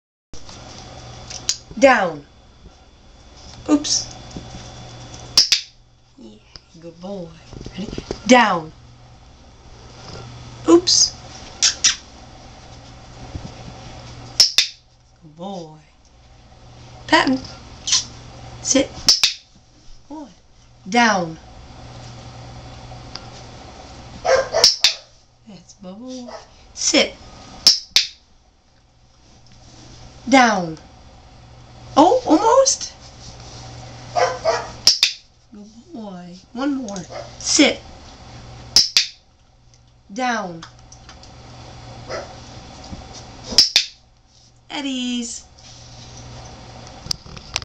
Hero Dogs Gen. George S. Patton - "down"
Hero Dogs "Patton" puppy in training, practices down.